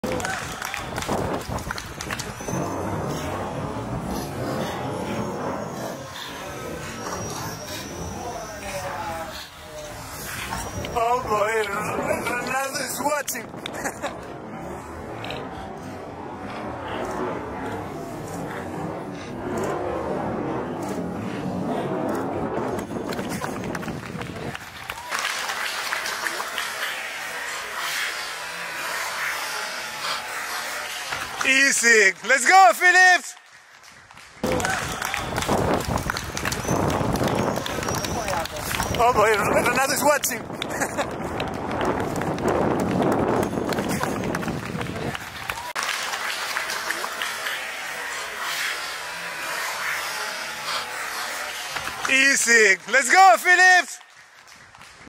PHILIPP PFLIEGER 10K 28:50 Berlin | Running Form
First race after 8 months and to come back with a win feels extra special! 🎉
Missed my 10k road PB by 1s but to be honest I am just super happy about my 28:50min and the way I ran it. 🙏
Felt so good to be back out there racing in Berlin with all the other runners, people alongside the course cheering us on and so many familiar faces. 🖤
Thank you Berlin for making this happen. That’s the way how I love our sport which is for everyone and I hope for all of us more of this will be possible again soon. 🙌